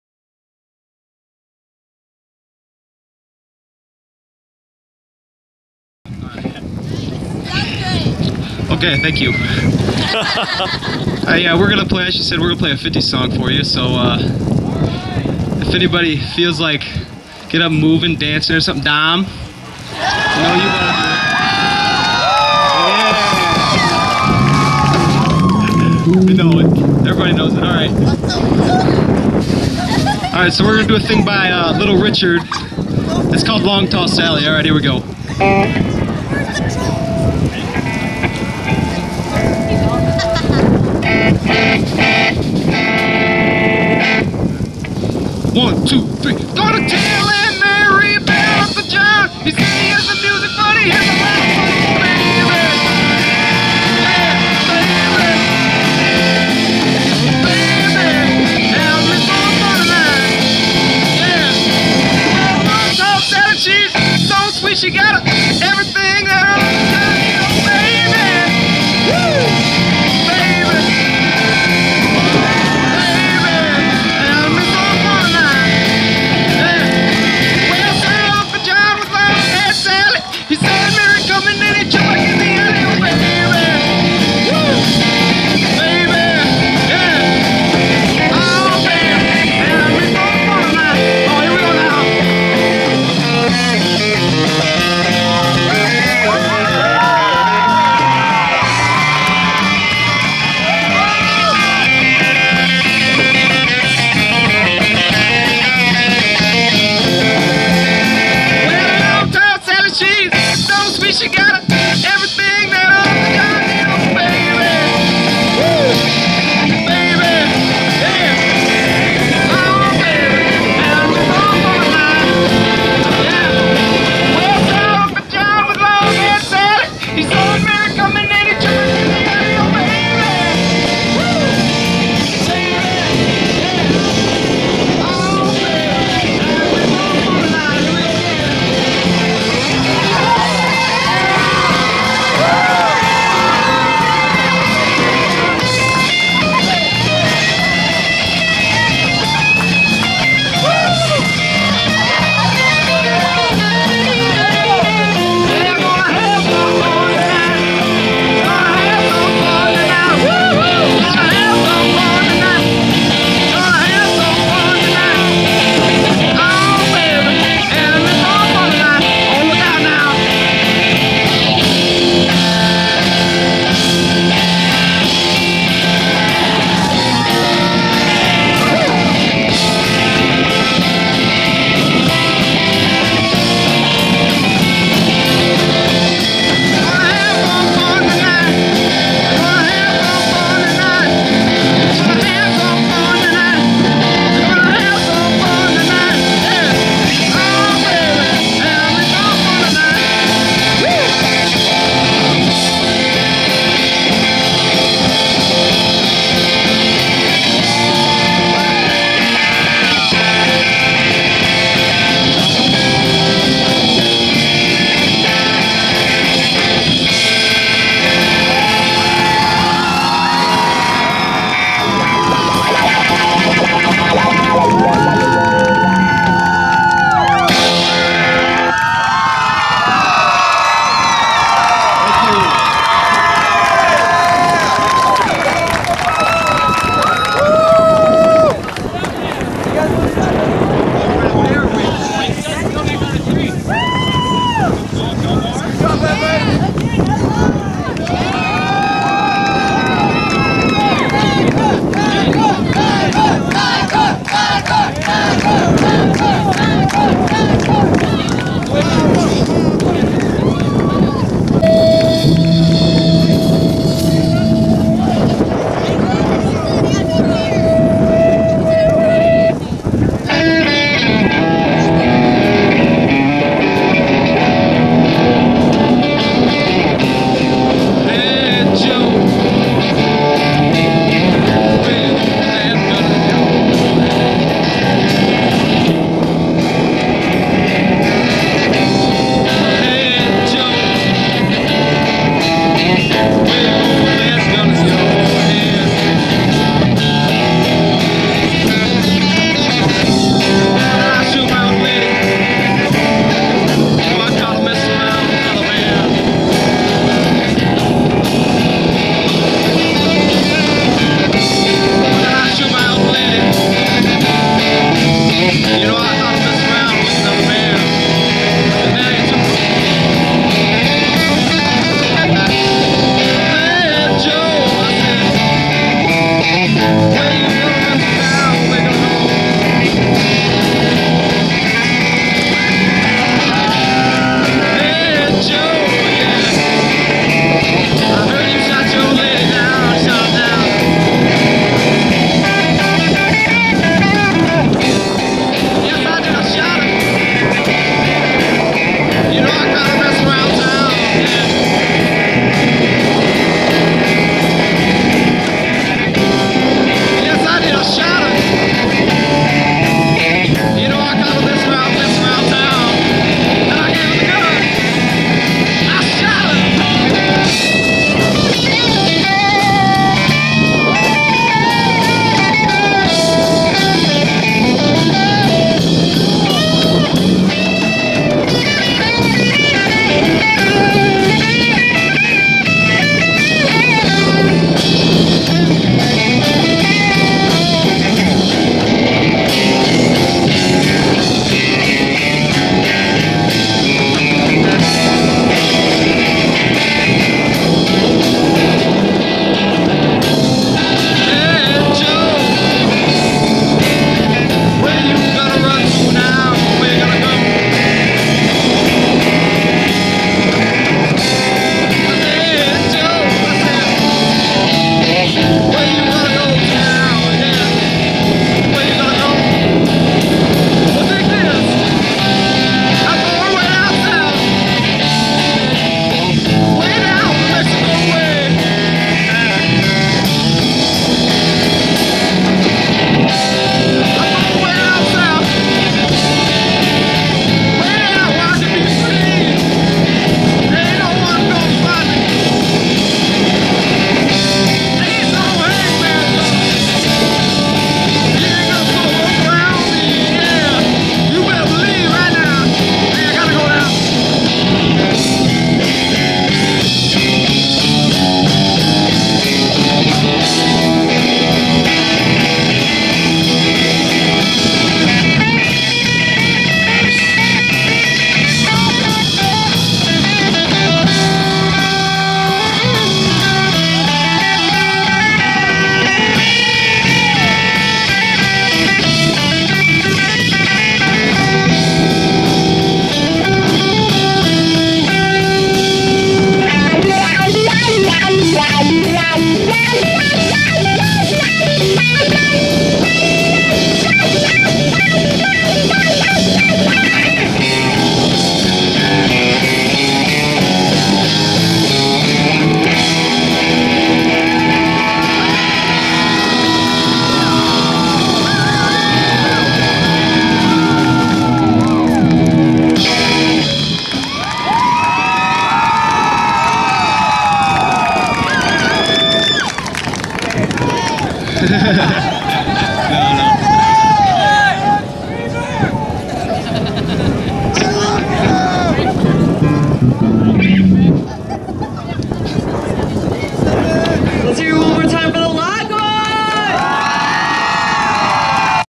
The Lockhorns - Homecoming 2012.mov
The Lockhorns! A group of students that performed for our 2012 Homecoming Assembly.